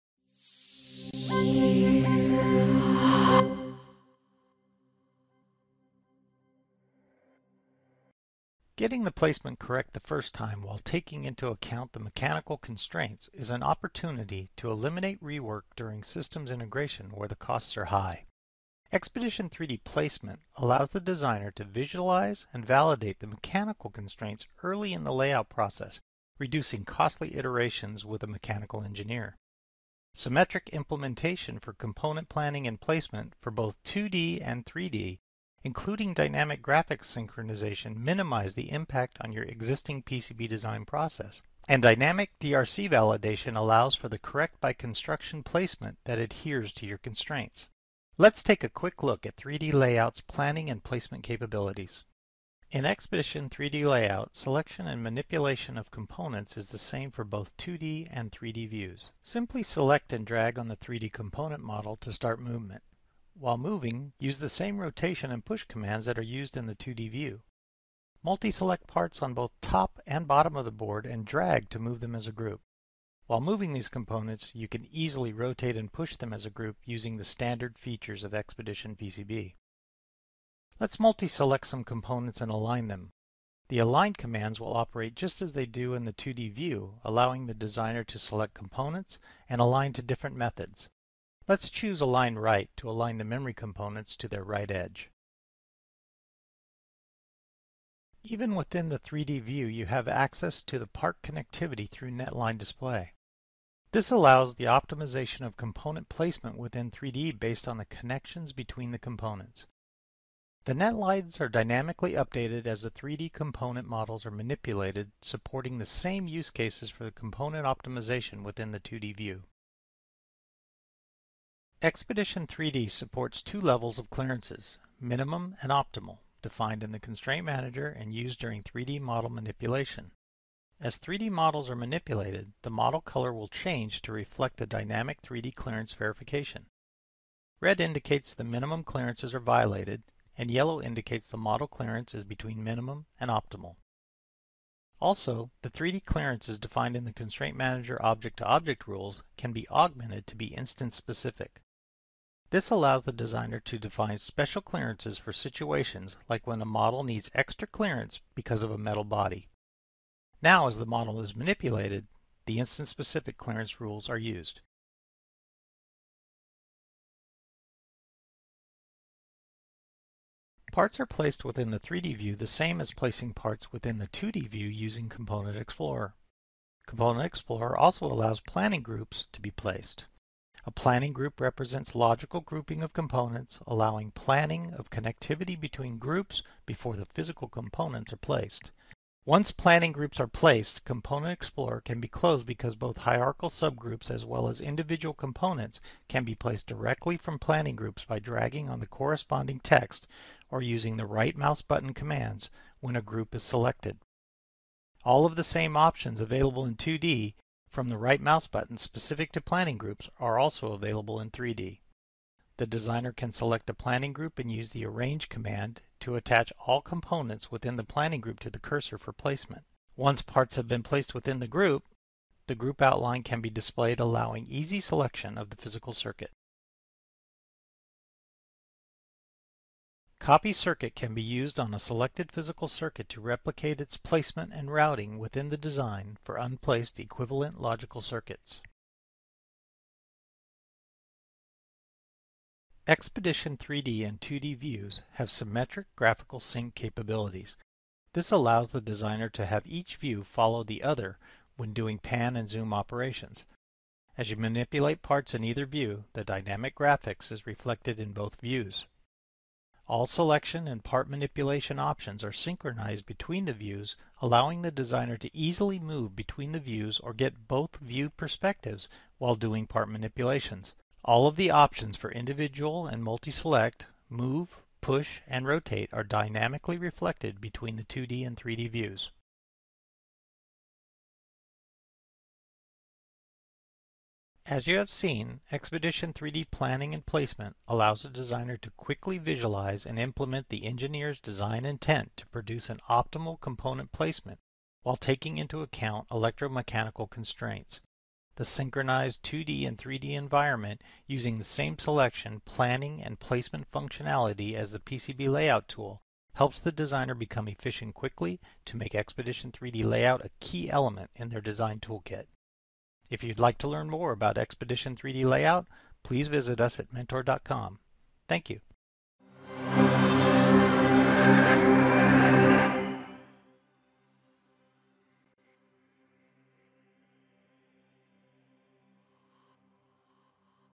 Xpedition xPCB: 3D Component Planning and Placement
Xpedition xPCB enables 3D component planning and placement while taking into account electro-mechanical constraints.